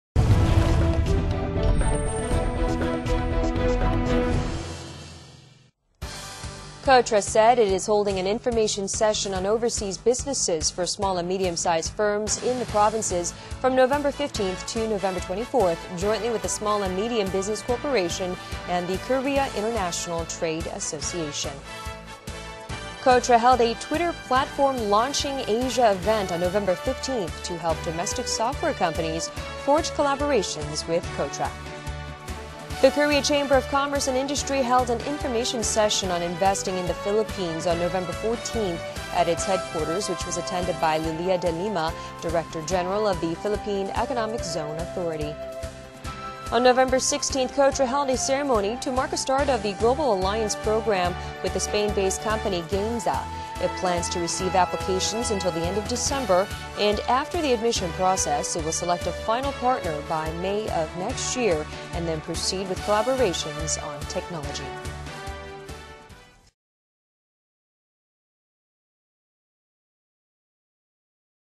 [Industry professional news channel itsTV] Weekly News Brief_1125
Export information session for SMEs outside Seoul

KOTRA said it is holding an information session on overseas businesses for small- and medium-sized firms in provinces from Nov. 15 to Nov. 24, jointly with the Small and Medium Business Corporation and the Korea International Trade Association.

KOTRA holds 'Twitter Platform Launching Asia'

KOTRA held a 'Twitter platform launching Asia' event on Nov. 15 to help domestic software companies forge collaborations with KOTRA.

Korea Chamber of Commerce hold info session on investing in Philippines

Korea Chamber of Commerce and Industry held an information session on investing in the Philippines on Nov. 14 at its headquarters, which was attended by Lilia De Lima, Director General of the Philippine Economic Zone Authority.

KOTRA joins hands with Gamesa from Spain

On Nov. 16, KOTRA held a ceremony to mark a start of the global alliance program with Spain-based company Gamesa. It plans to receive applications until the end of December and after the admission process, it will select a final partner by May next year and then proceed with collaborations on technology